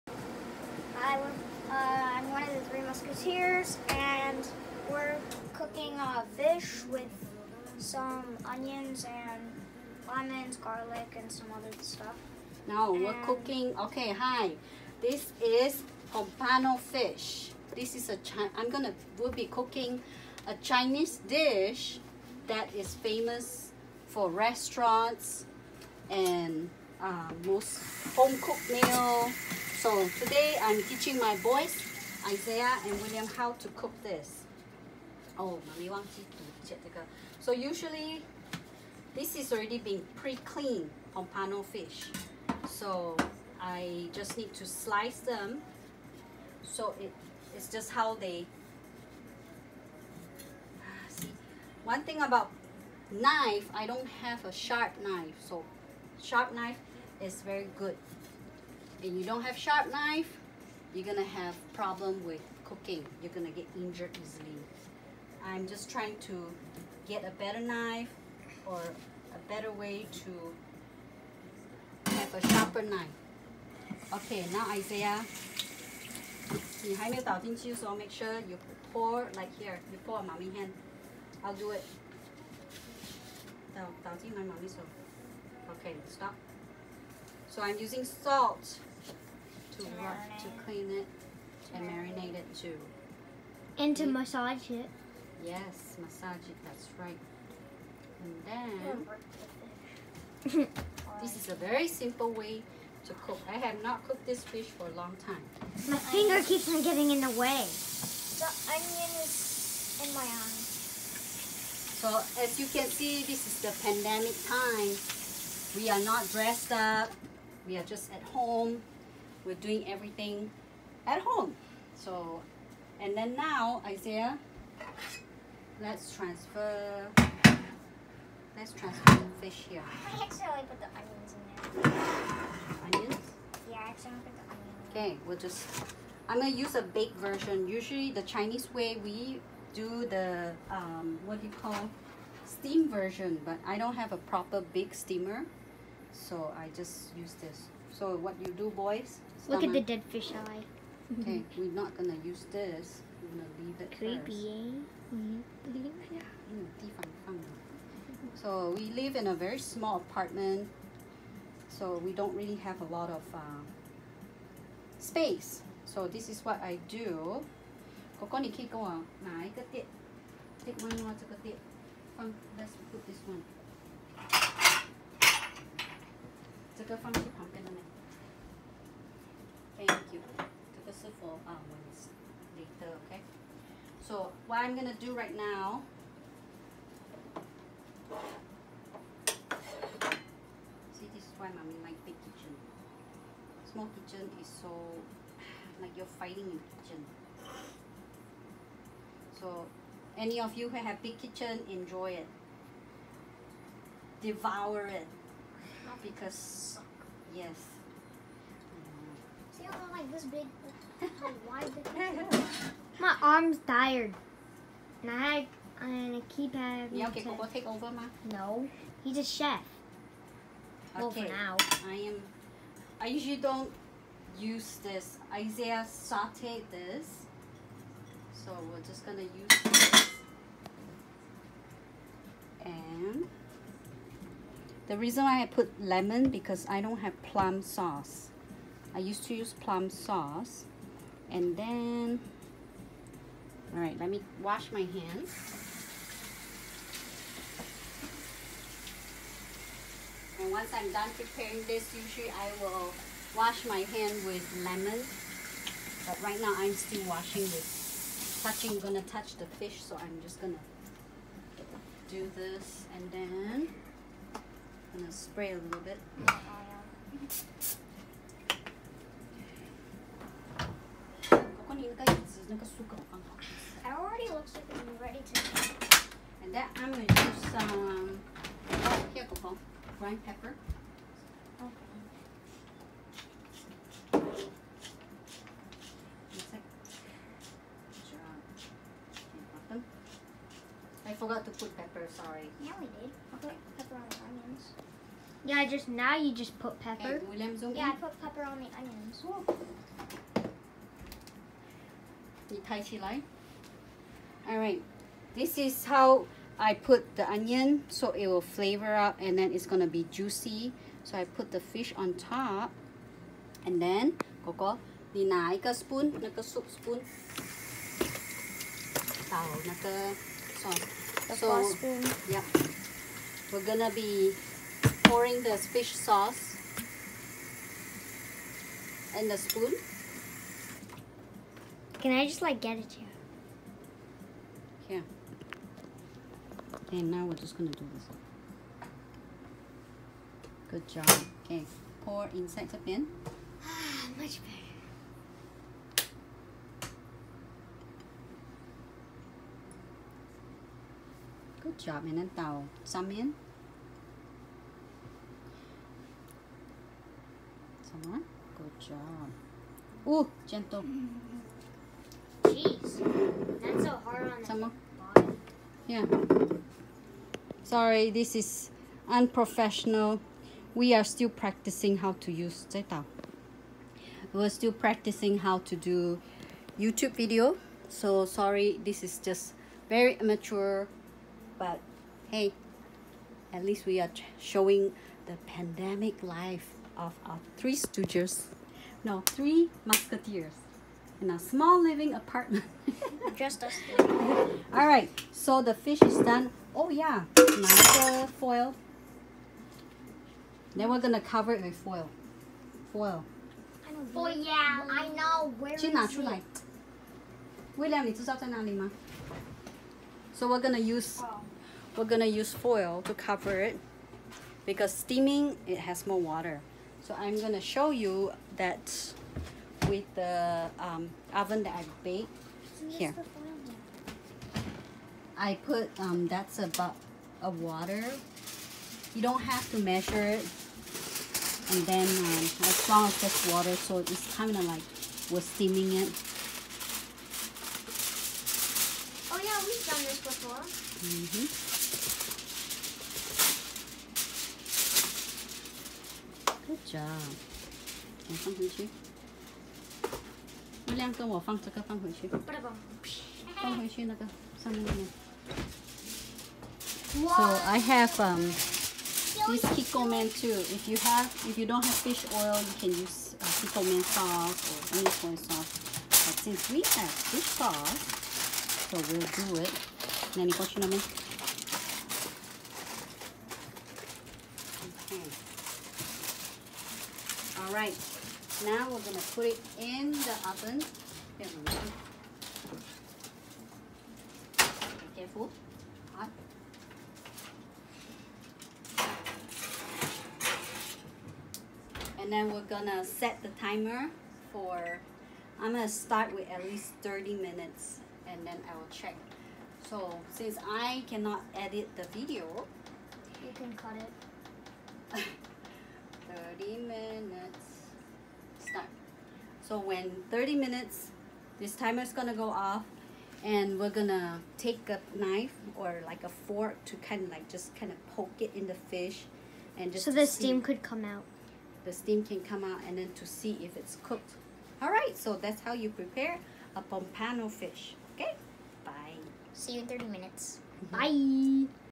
How to cook Pompano fish part 1
We are making Pompano fish me and my 11 year old son, and my 10 year old son are helping me make Pompano fish.